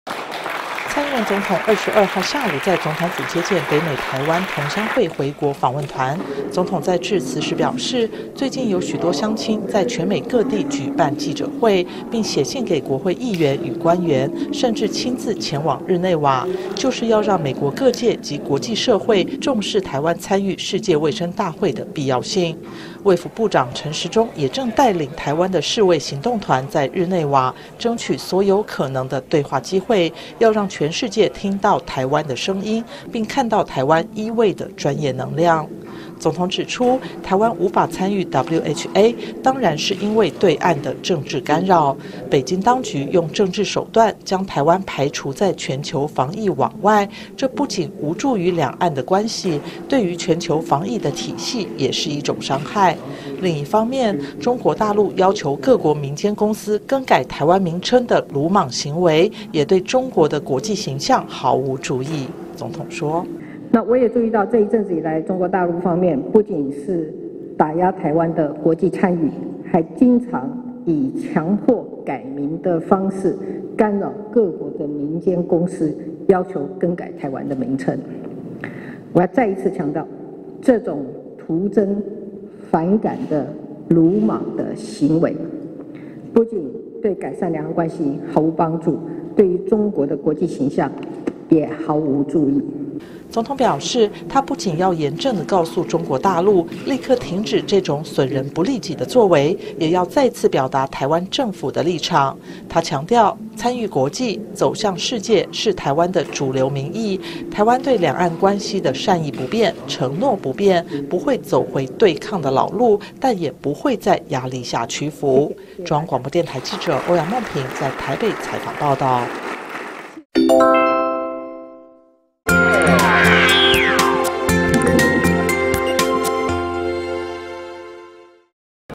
中國施壓外商改名 總統：魯莽行為徒增反感【央廣新聞】
蔡英文總統22日接見「北美台灣同鄉會回國訪問團」時表示，北京當局用政治手段，將台灣排除在全球防疫網之外，不僅無助兩岸關係，也傷害全球防疫體系。總統並指出，中國大陸這段期間強迫各國民間公司更改台灣的名稱，這種徒增反感的魯莽行為不僅無助於改善兩岸關係，也對中國的國際形象毫無助益。(記者歐陽夢萍採訪報導)